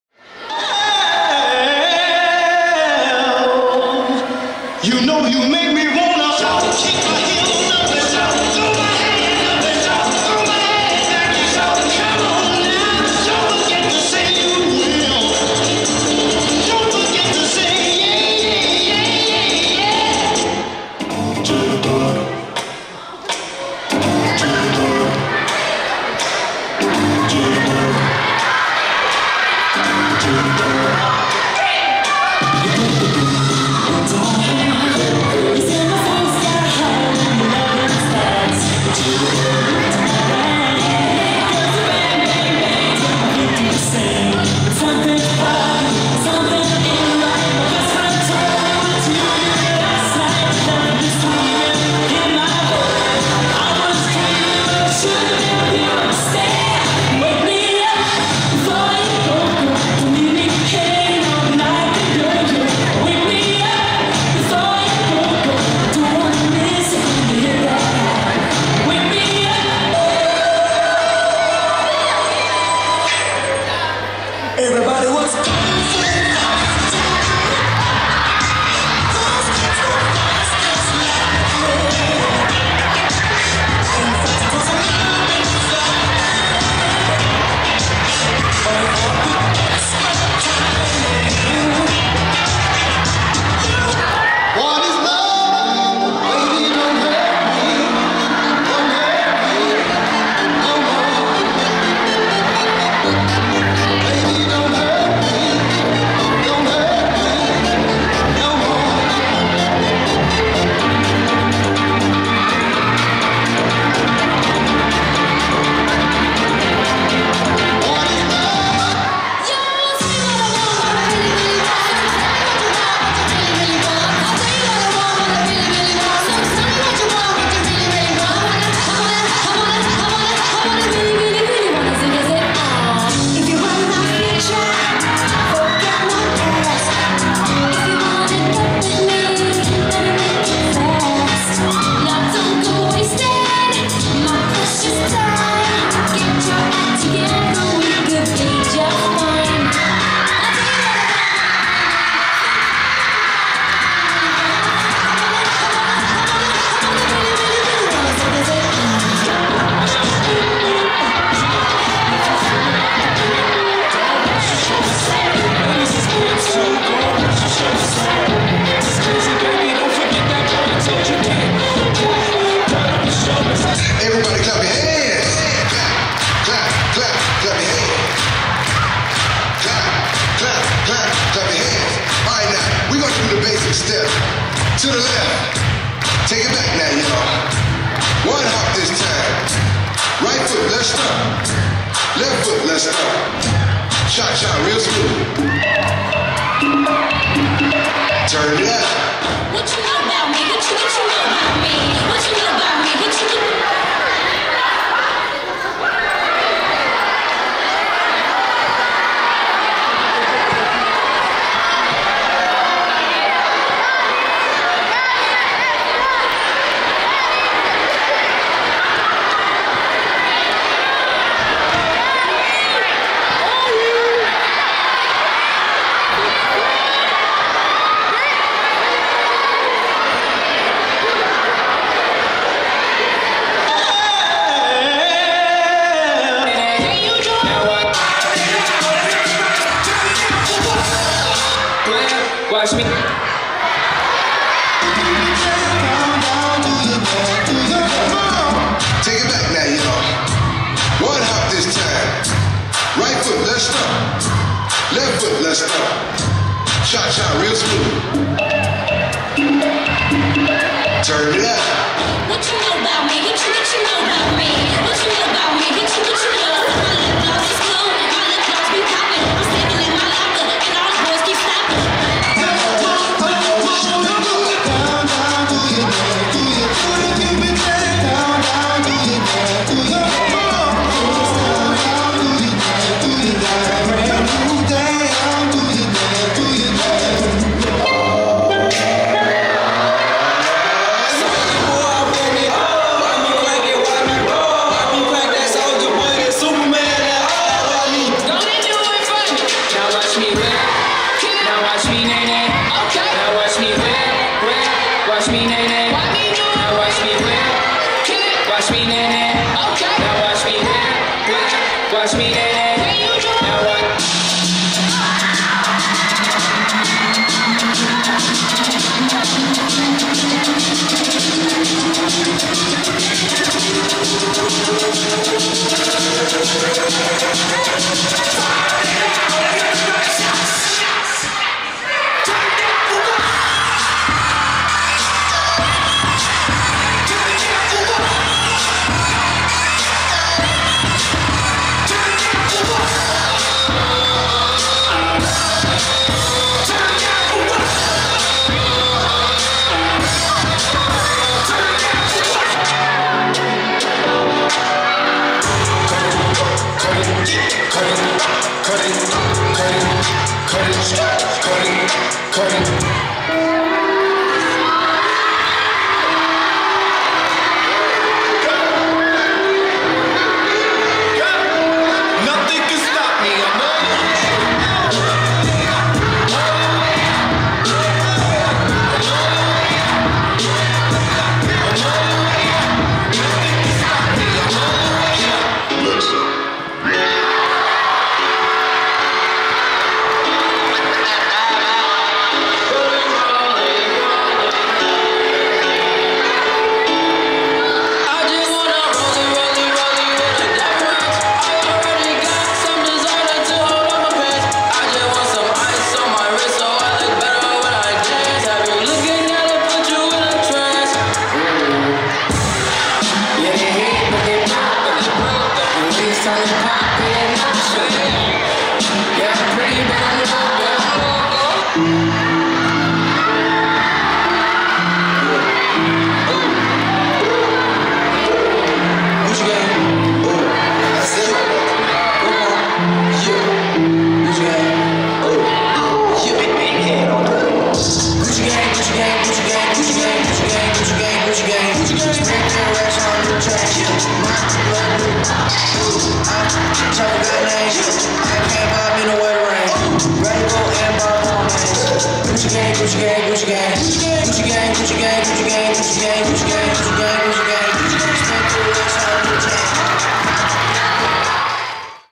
Stetson Holiday Show Dance Compilation 6th Grade Team
Please enjoy this performance from the Stetson Holiday Show organized by Mr. Shayka.